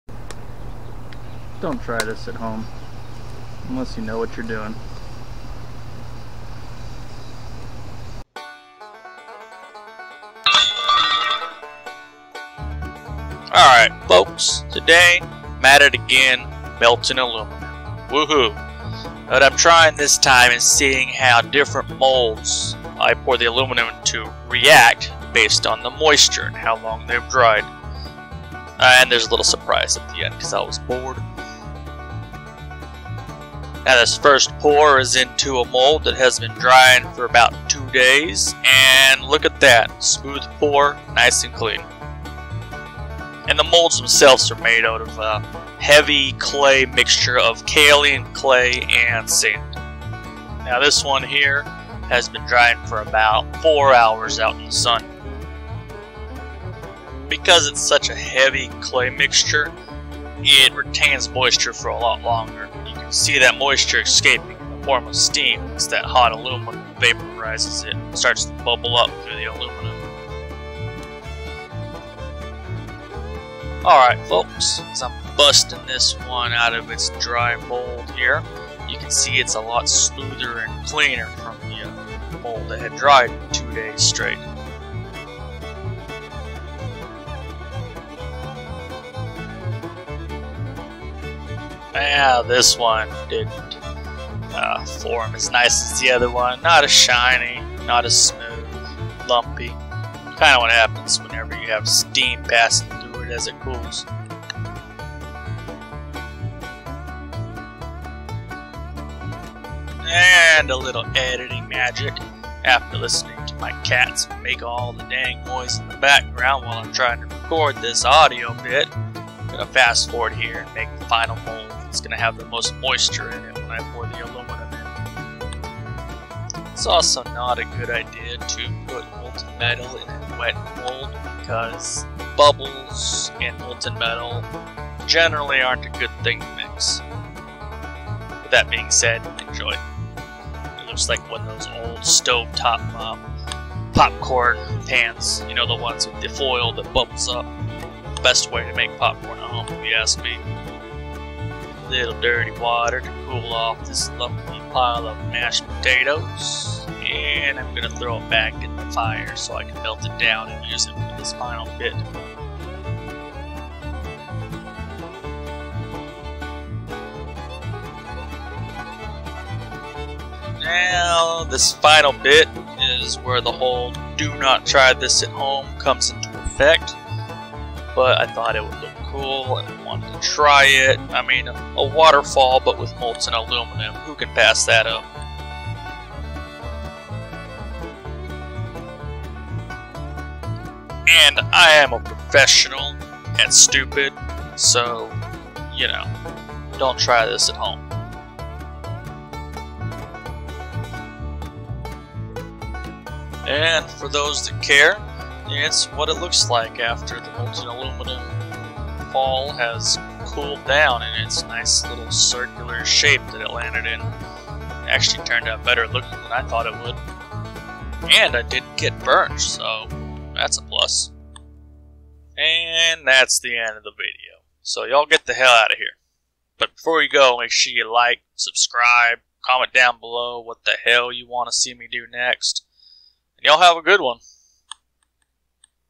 Molten Metal vs Moisture during a pour.
I melted aluminum and poured it into different clay/sand molds at different levels of dryness to observe how the molten metal reacted to moisture... then i made a waterfall type structure but with molten metal...